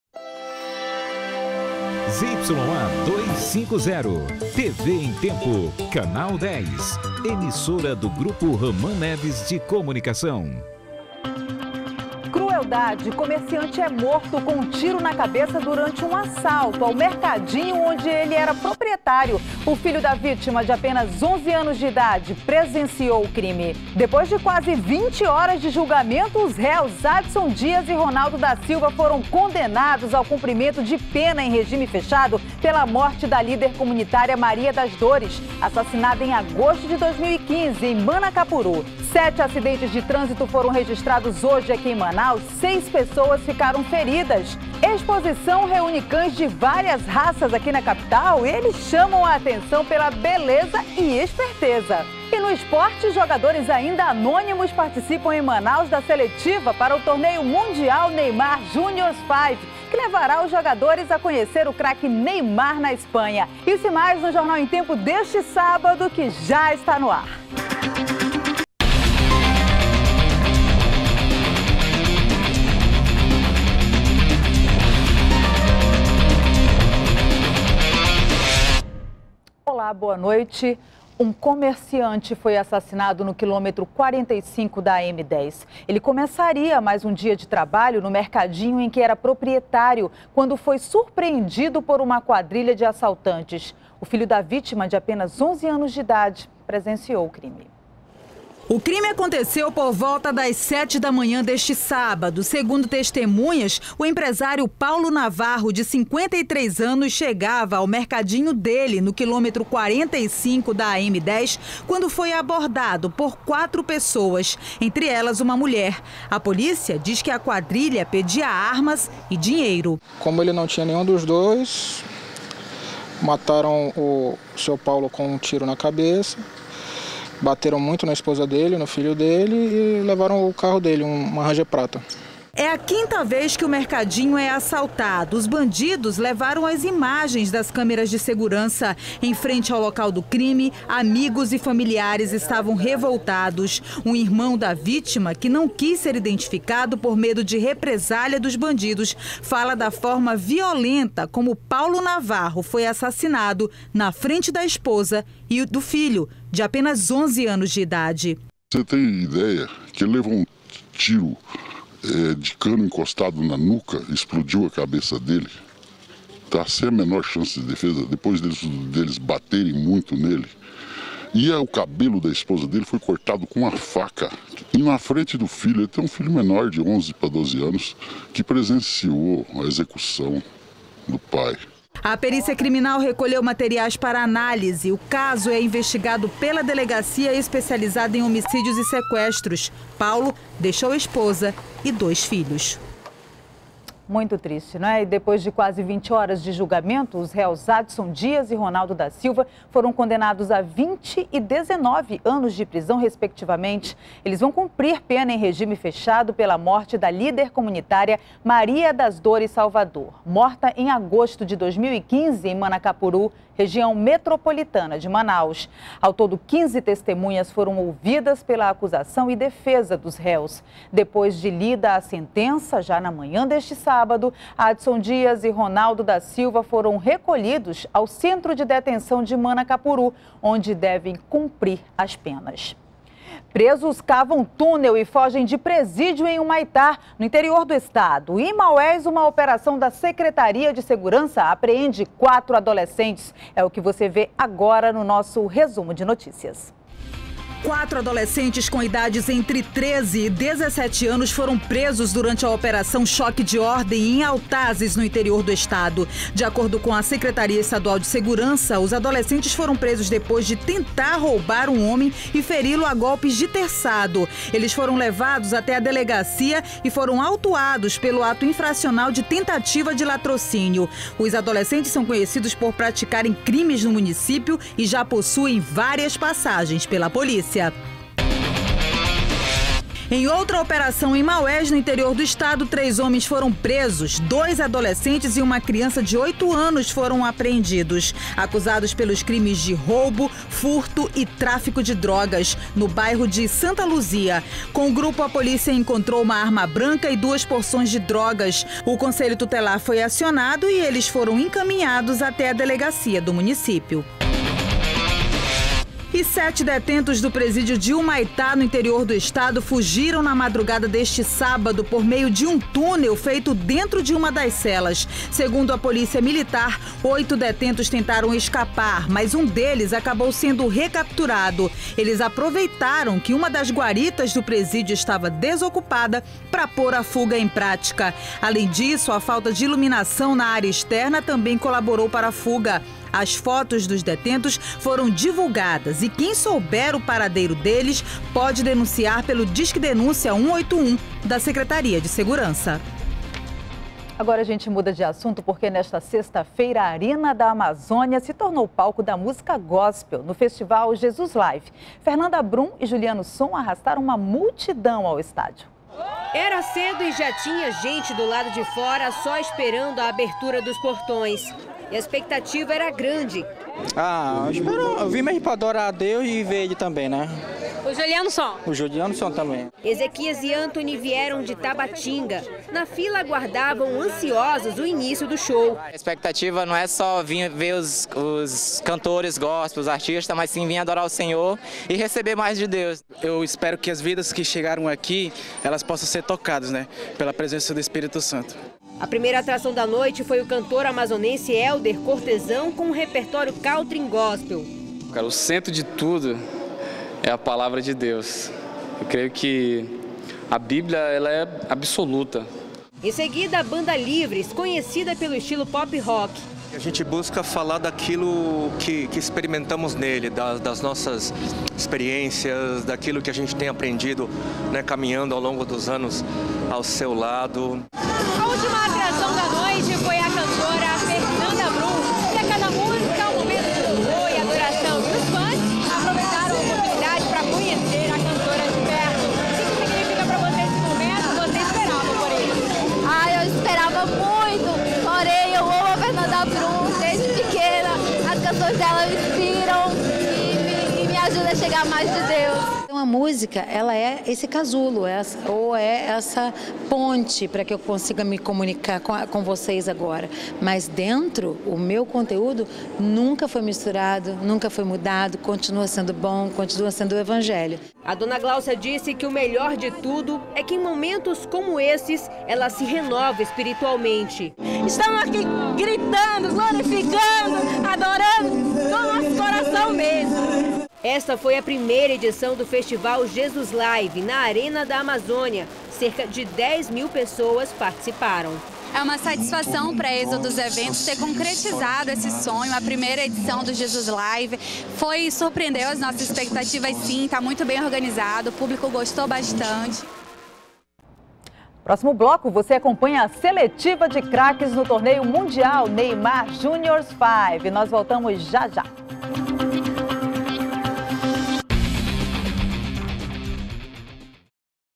Comerciante é assassinado no quilometro 45 da AM-010
01º BLOCO JORNAL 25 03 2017 - Comerciante é assassinado no quilometro 45 da AM-010